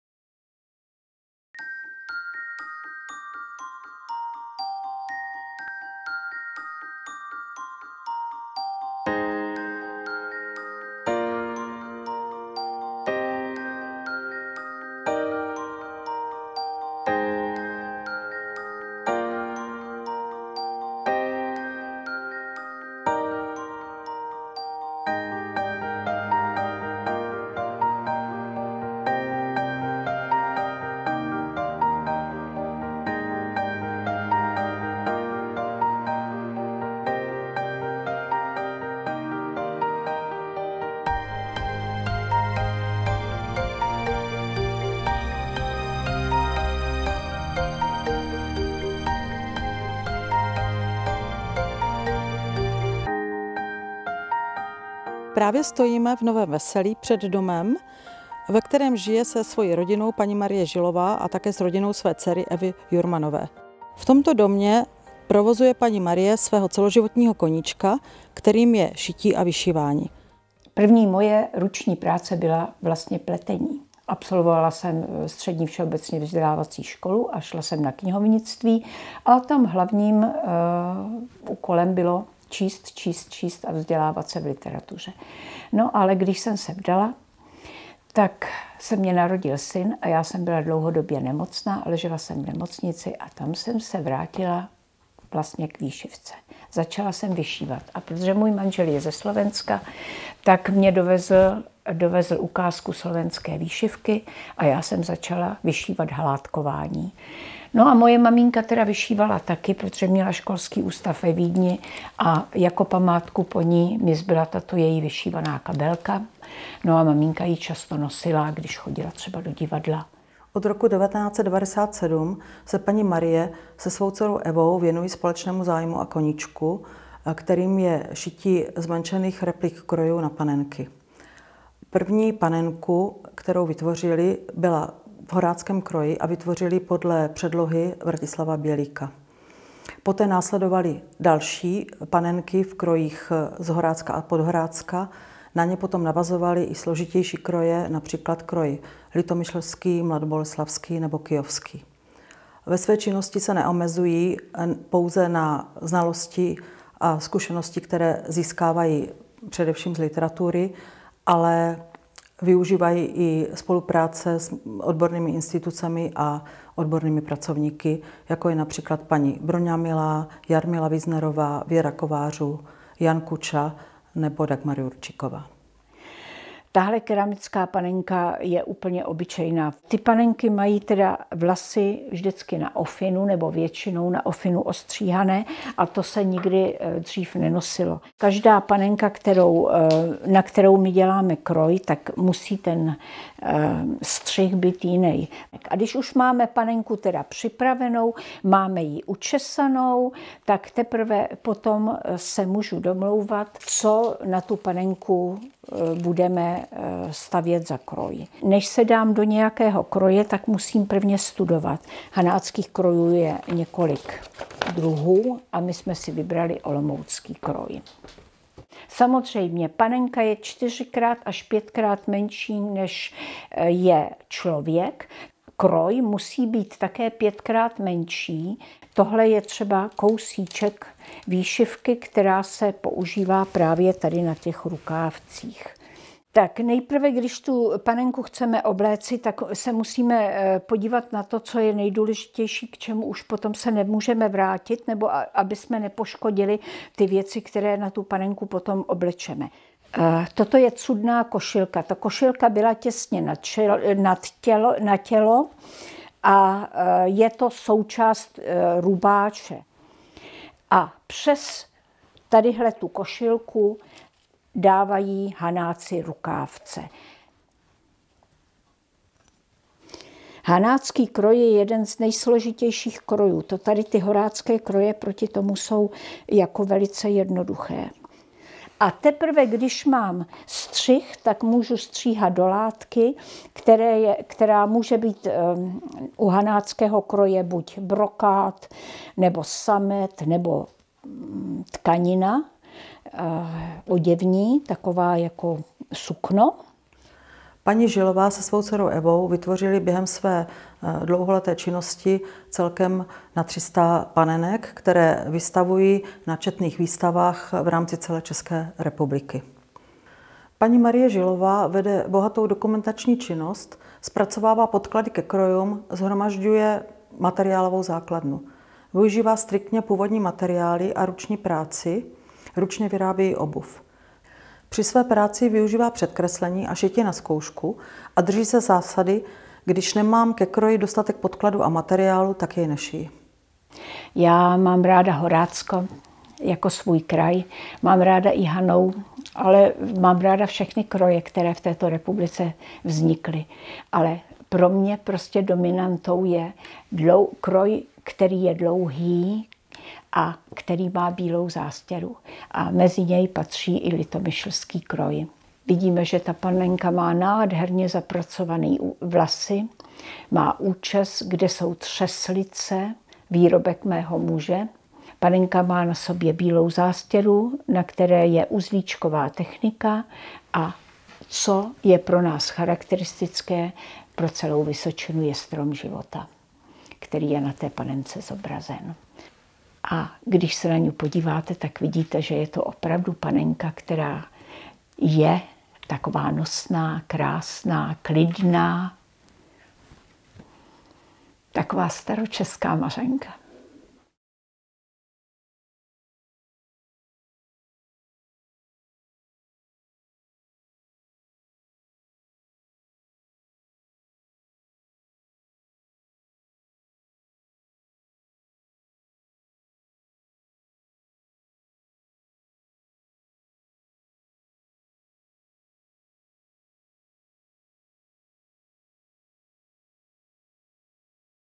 Marie Žilová autorka modelů a replik krojů z Nového Veselí
Zastupitelstvo Kraje Vysočina udělilo v roce 2018 paní Marii Žilové titul Mistr tradiční rukodělné výroby Kraje Vysočina.